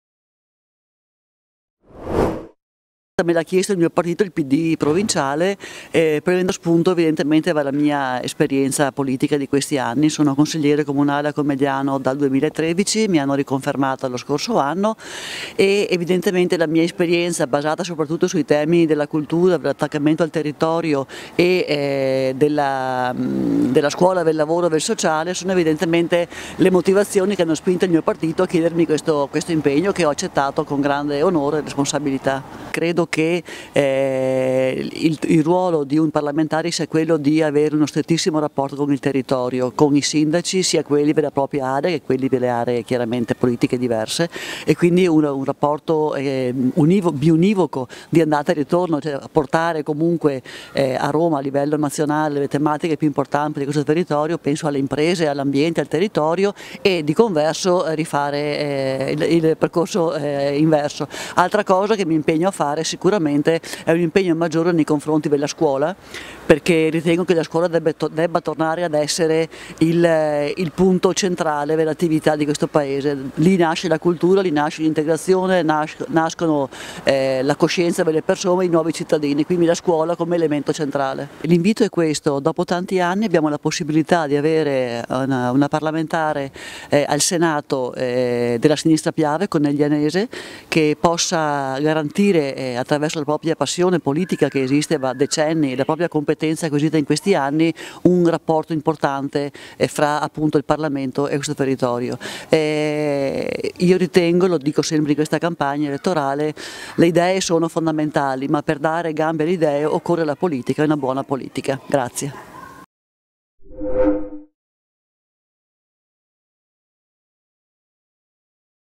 Politiche 2018, i candidati in pillole: Isabella Gianelloni (Pd) di Conegliano
Isabella Gianelloni, di Conegliano è candidata al Senato della Repubblica con il Partito democratico, nel collegio uninominale di Treviso.

Docente di lettere alla scuola media, attualmente consigliere comunale a Conegliano, la 56enne Gianelloni ha una grande passione per la storia e ha scritto diversi saggi e romanzi.